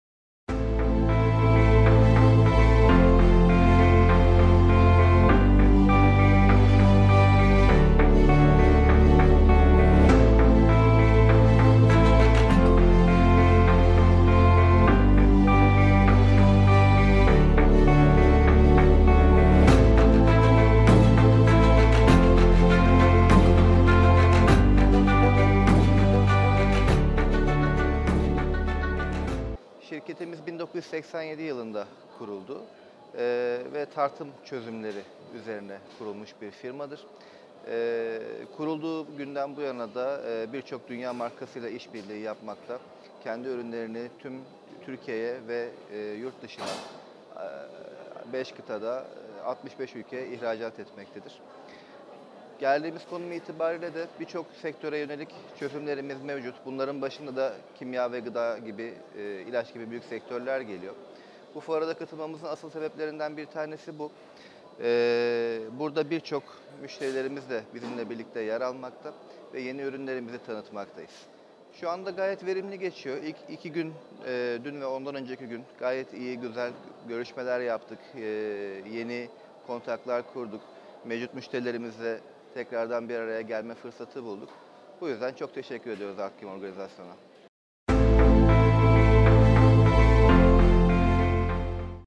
Turkchem Kimya Sanayi Fuarındayız...
8-10 Kasım 2018 tarihlerinde katılmış olduğumuz TURKCHEM Uluslararası Kimya Sanayi Fuarını başarılı bir şekilde tamamladık.